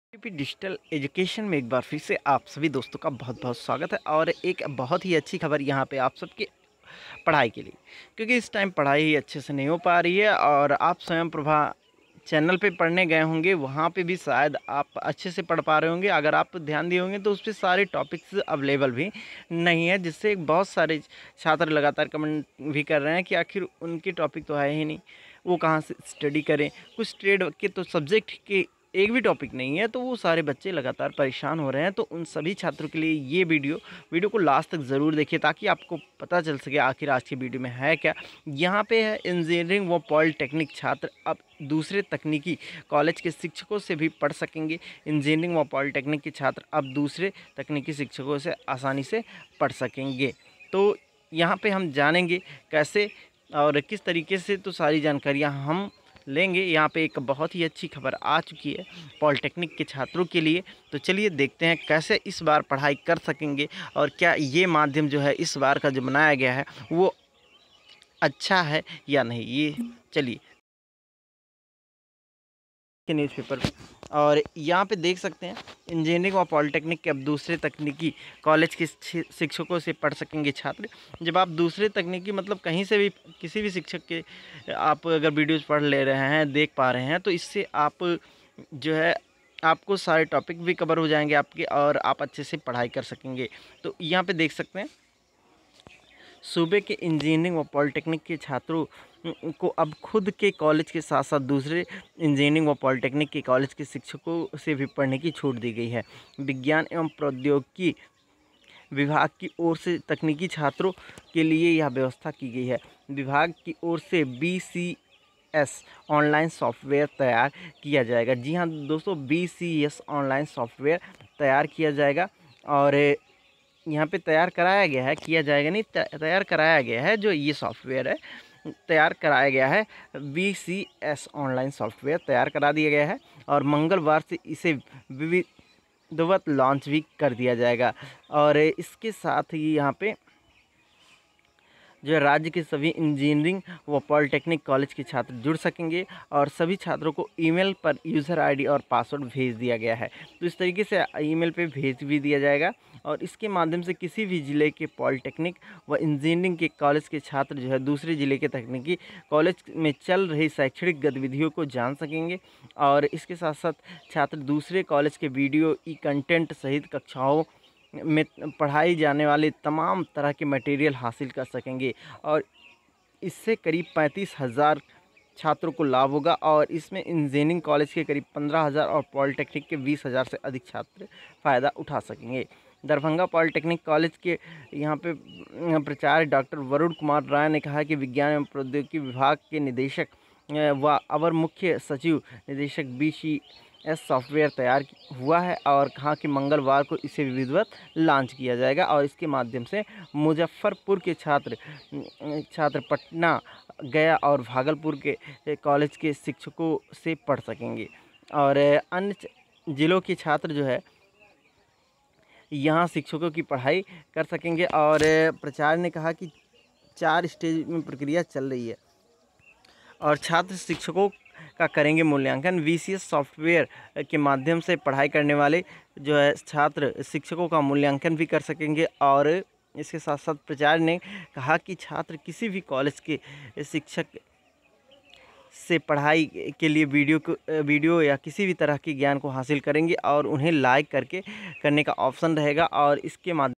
SBTE इंजीनियरिंग व पॉलीटेक्निक छात्र अब वीसीएस ऑनलाइन सॉफ्टवेयर से कर सकेंगे पढ़ाई
sbte बिहार के स्टूडेंट्स के लिए
इंजीनियरिंग व पॉलीटेक्निक छात्र अब वीसीएस ऑनलाइन सॉफ्टवेयर से कर सकेंगे पढ़ाईइंजीनियरिंग व पॉलीटेक्निक छात्र अब वीसीएस ऑनलाइन सॉफ्टवेयर से कर सकेंगे पढ़ाई